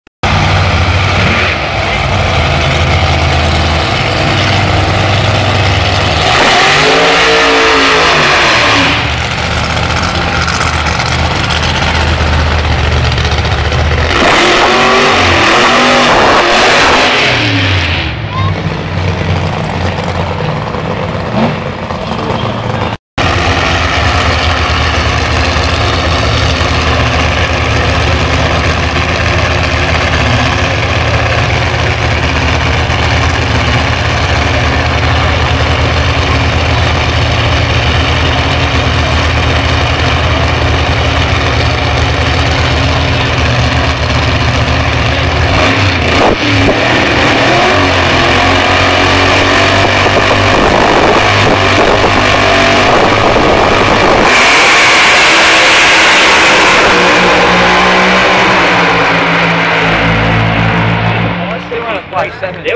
Debbie Reid and Steven Ham at Top Alcohol round 2
Debbie Reid and Steven Ham at Top Alcohol round 2 track championship round 3 and top fuel round 3 sydney dragway